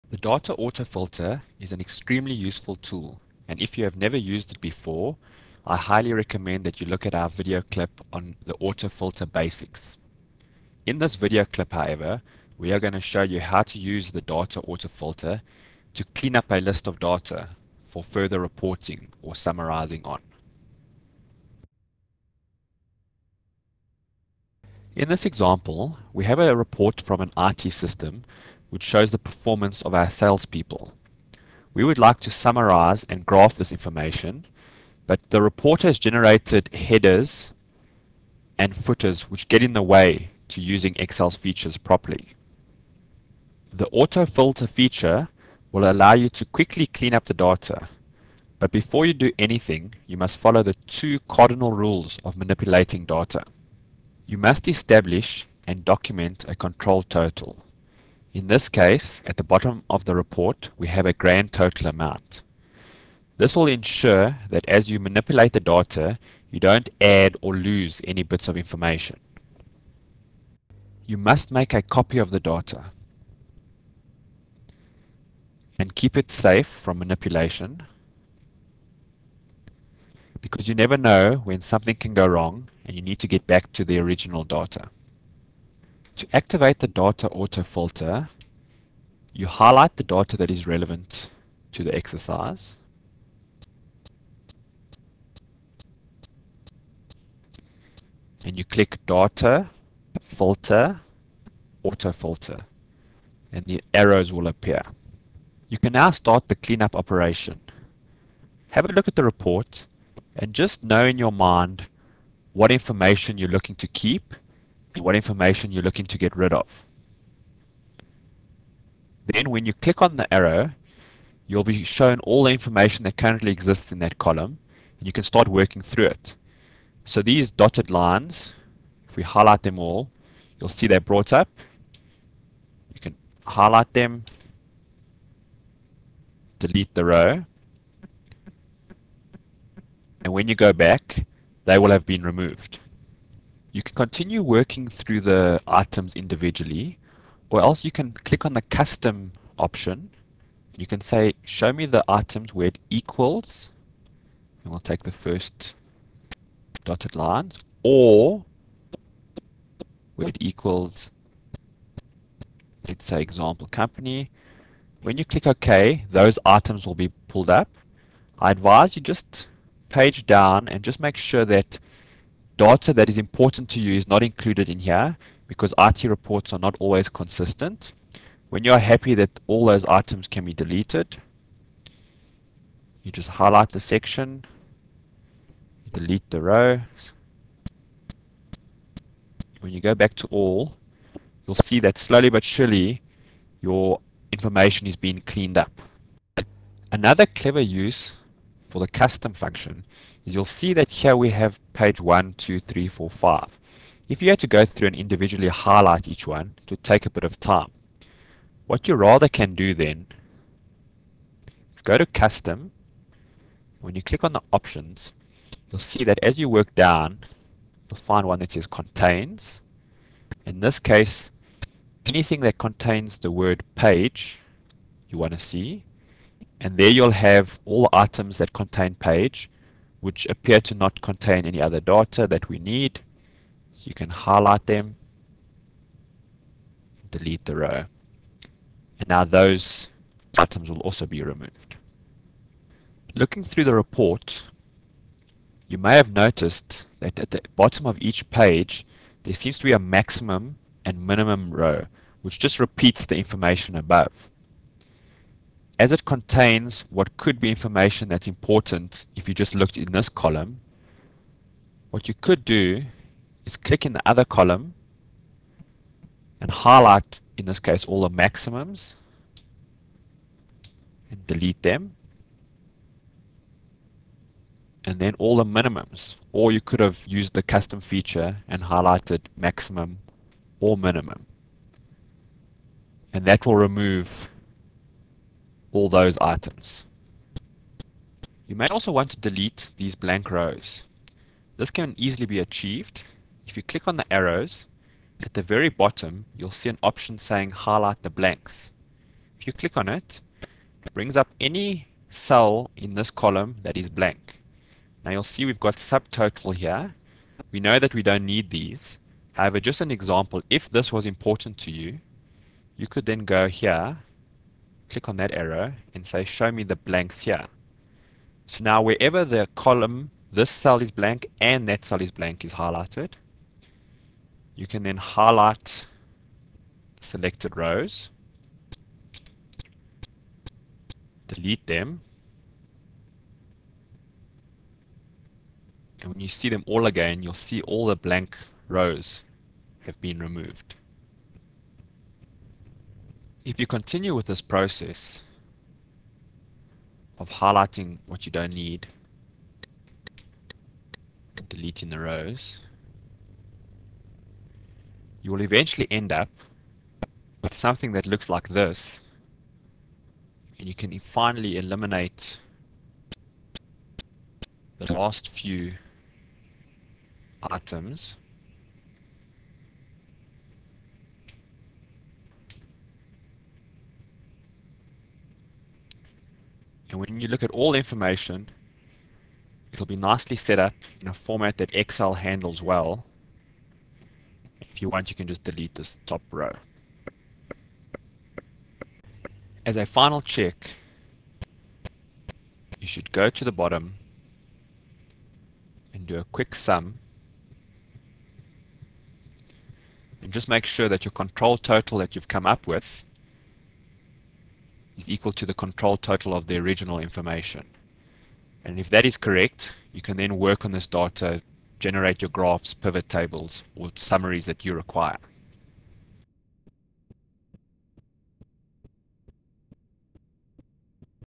Easily remove obsolete rows in Excel 2003 to create a database out of a report.
How to clean data using the Data Autofilter